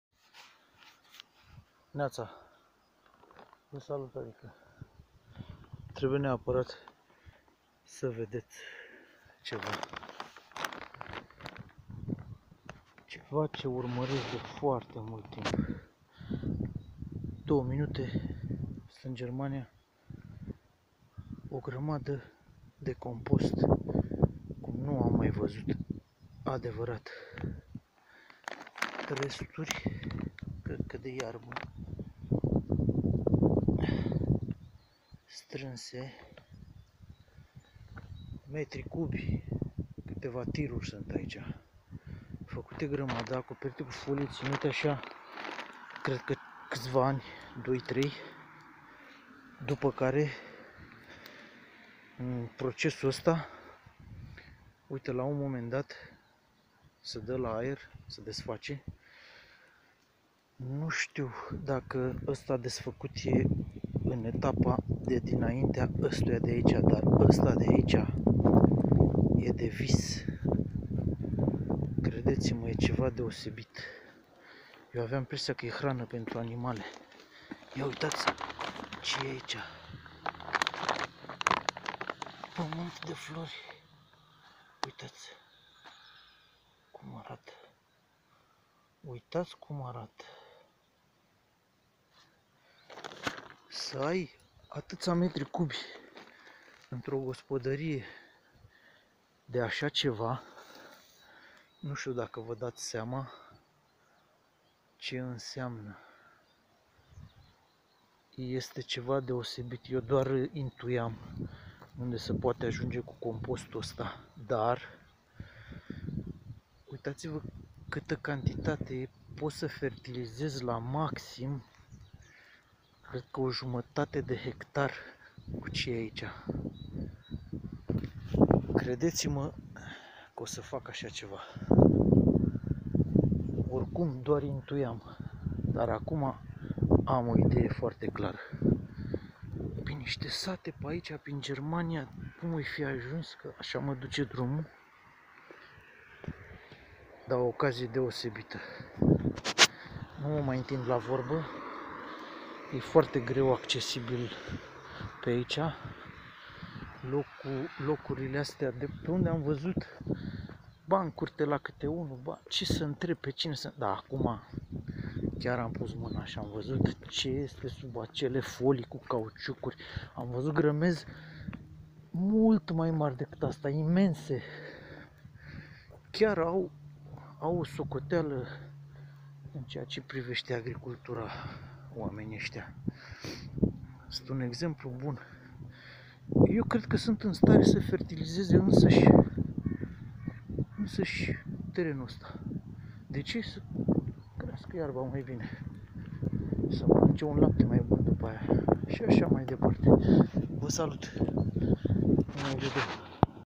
Compostul!!! Agricultură adevărată!!!
Acest produs ,este rezultatul transformării pe cale naturală a resturilor de origine vegetală,în minerale și compuși organici direct accesibile plantelor și este  munca a miliarde de bacterii de toate felurile timp de 1-2sau3ani.El este produsul ce omul l-a înlocuit cu îngrășămintele chimice în ultima suta de ani și pe care niciun producător MARE de produse agricole,nu își permite să îl folosească ca singură sursă de fertilizare a pământului.Doar producătorii mici își permit să utilizeze așa ceva!!!Gîndiți-vă la fiecare legumă sau fructă,care ajunge pe masa dv. în oricare zi,pentru o clipă,și la originea ei clară!De cele mai multe ori doar pare cunoscută însă în realitate nu știm nici măcar ce culoare are pămîntul în care au crescut,dacă acesta există!!!Încercarea la nivel mondial,este de a face din gustul natural al produselor agricole,o necunoscută!Noi mai știm de cînd eram mici dar copii noștri nu vor mai avea termen de comparație dacă nu știm CE ESTE COMPOSTUL!?!?